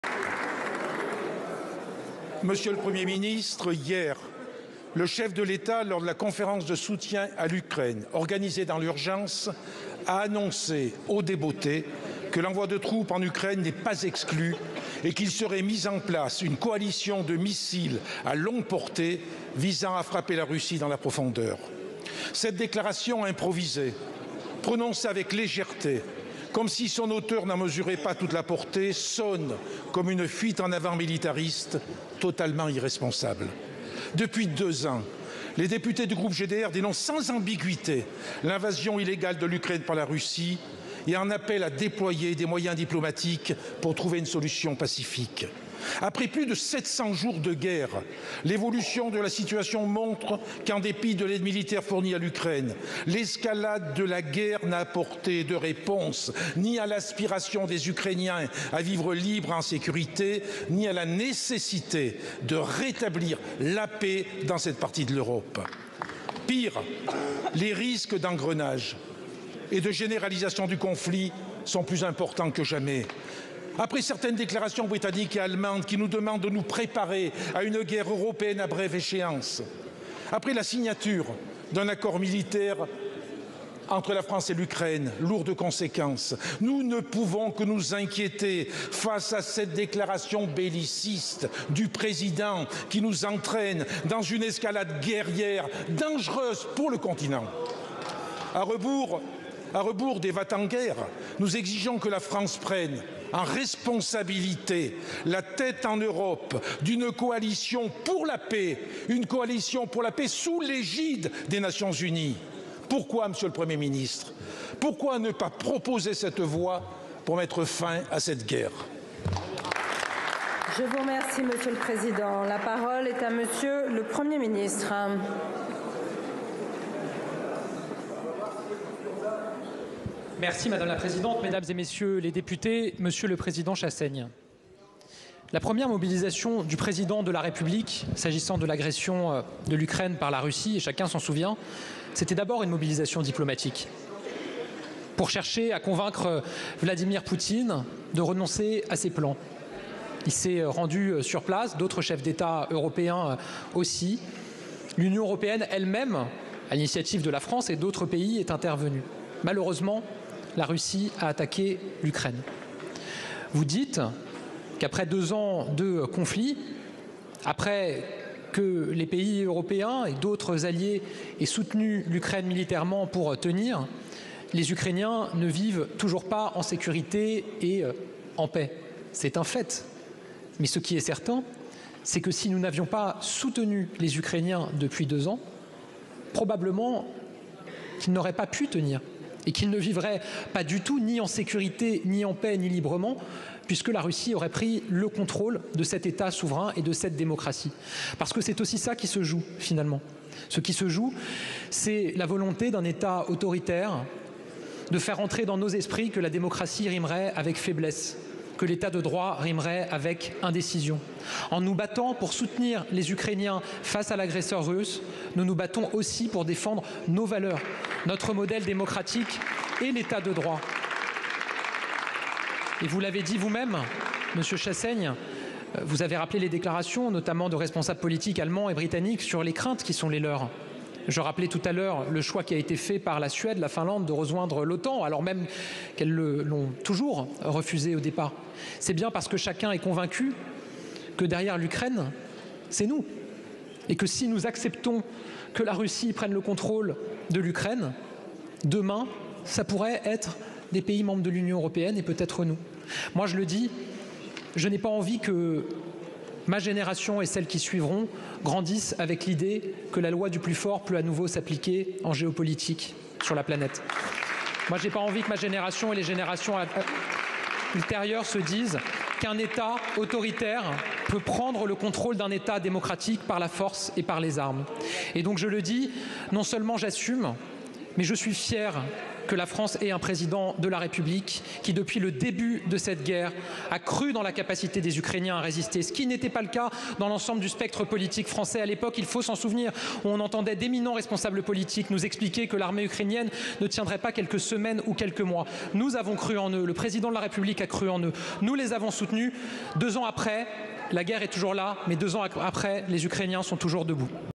Ukraine Urgence d'une coalition pour la paix !
🔴 Urgence d'une coalition pour la paix ! 🔴
➡️ "Nous ne pouvons que nous inquiéter face à cette déclaration belliciste du Président qui nous entraîne dans une escalade guerrière dangereuse pour le Continent..." Andre Chassaigne, Député PCF du Puy-de-Dôme